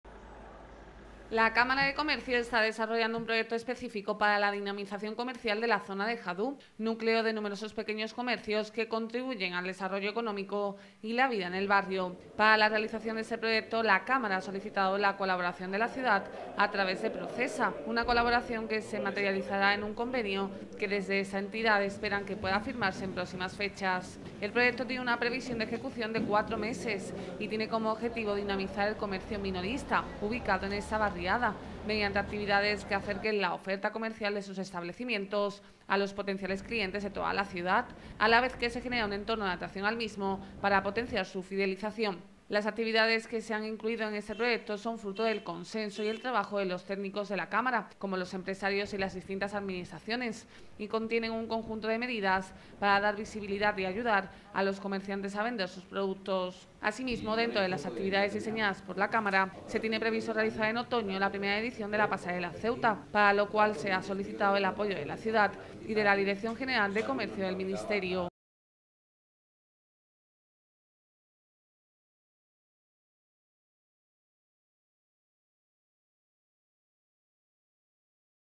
La Cámara elabora un proyecto de dinamización del comercio en Hadú
La Cámara de Comercio ha elaborado un conjunto de actividades y proyectos para dinamizar y fomentar el comercio y la actividad comercial en la zona de Hadú. Para este proyecto la Cámara ha solicitado la colaboración de la Ciudad a través de PROCESA, una colaboración que se plasmará en un convenio que esperan que pueda firmarse en próximas fechas.